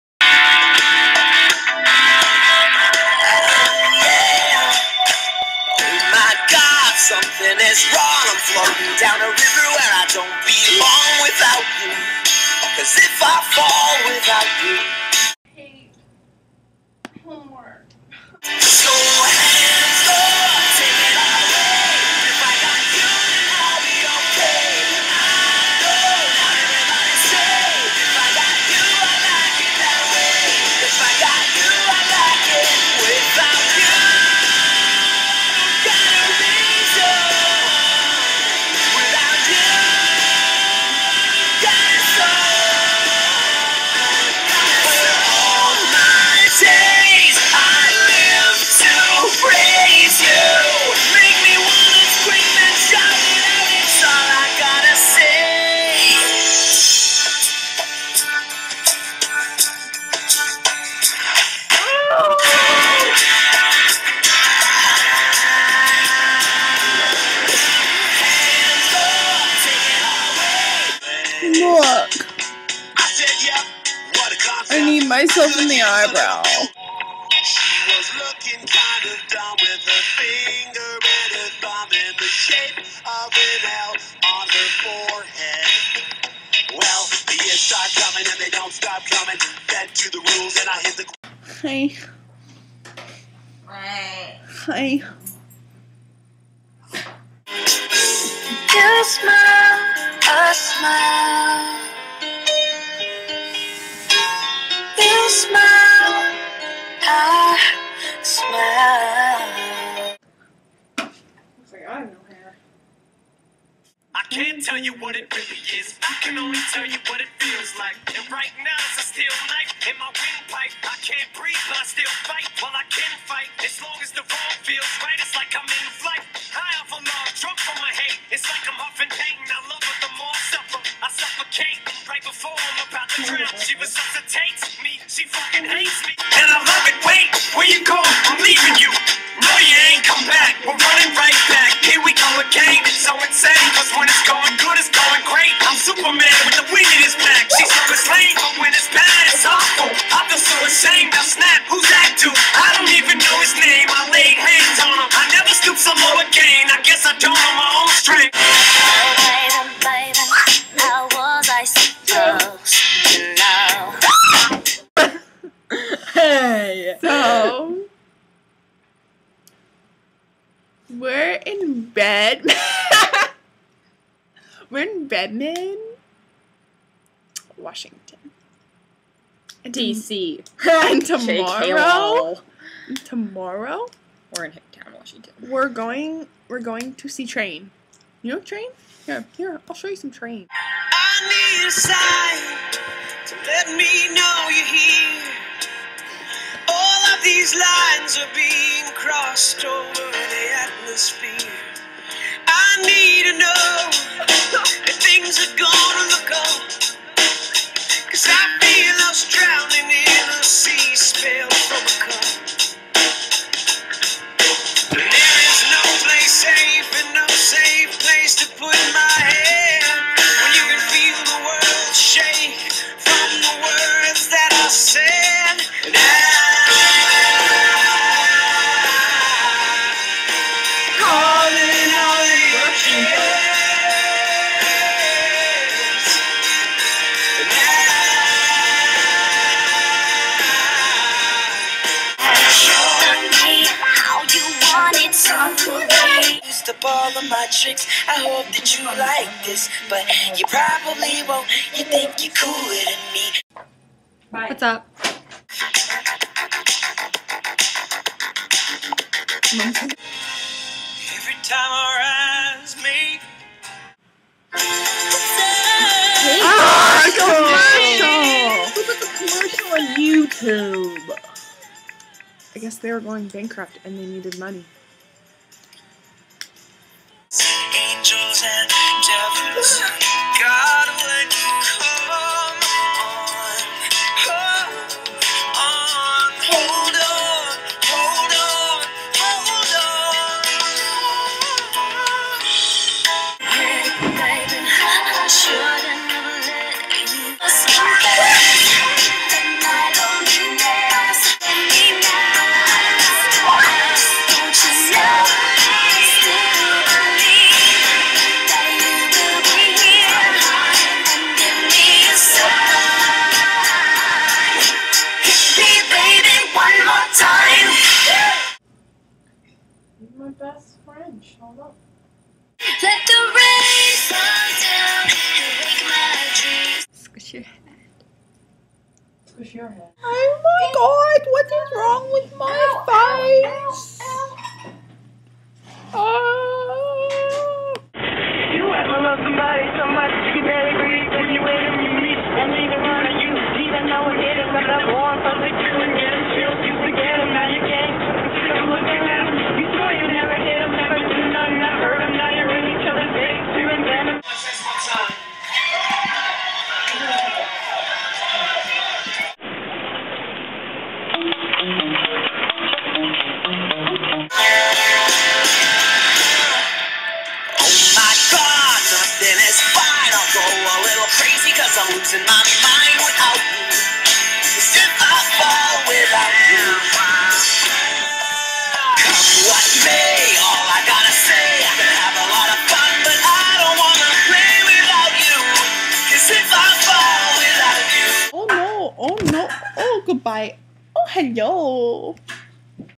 A WEEKEND IN REDMOND WASHINGTON
Trip to Redmond, WA with my friend Kayley for a Train concert!